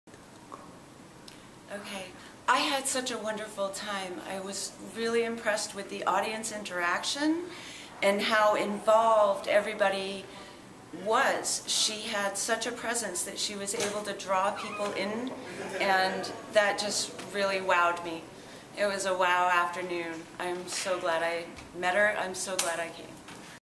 Lia Marie recomands Sharon Aizen and her great lecture "Secrets of a winning mindset"
יוצרים תוצאות עם שרון אייזן
ליה מארי ממליצה על שרון אייזן